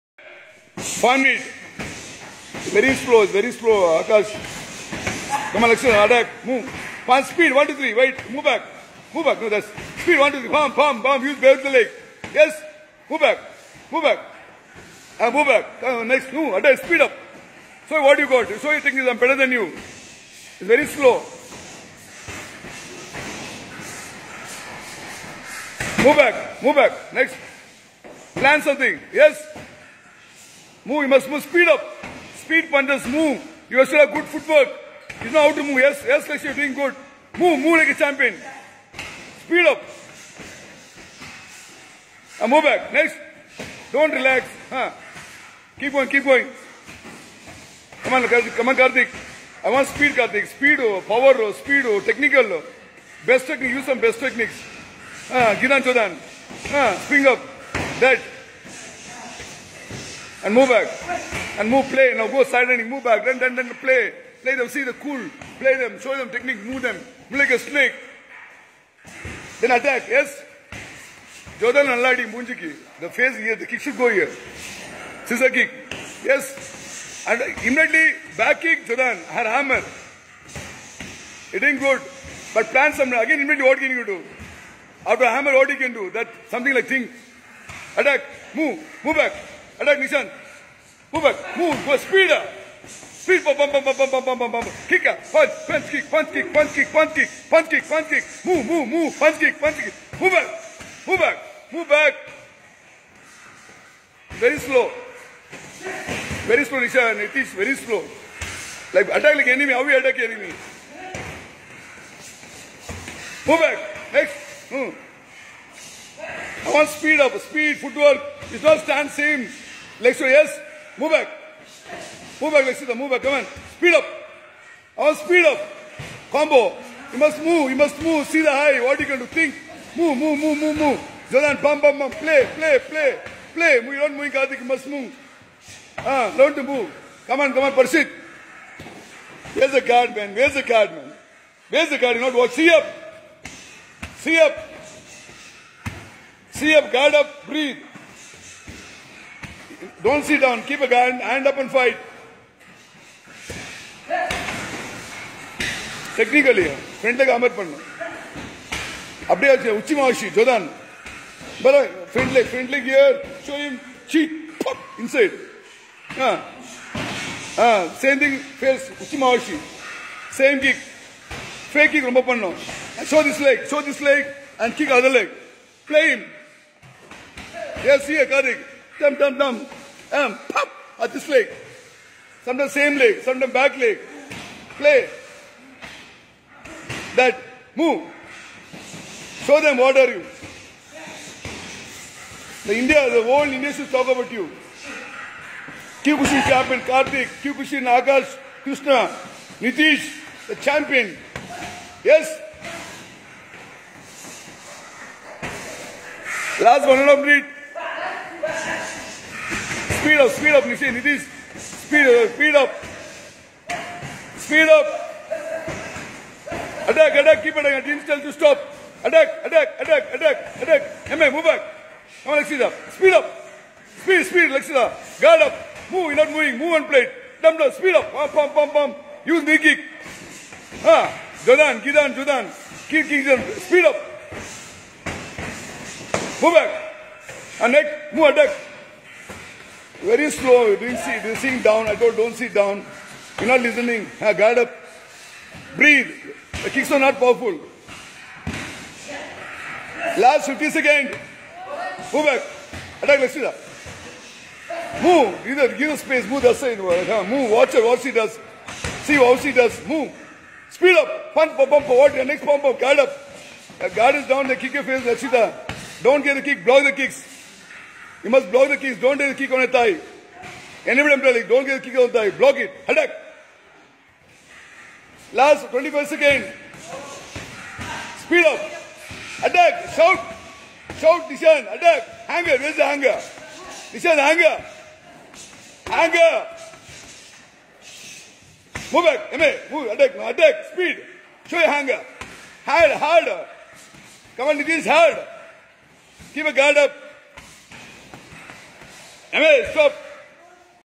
Kyokushin Karate(3)
IKO Matsushima Kyokushin Karate India 🇮🇳
30th December 2019 Training very hard for the South India Kyokushin Karate Tournament Tirupur Tamil Nadu on January 5th 2020
Osu...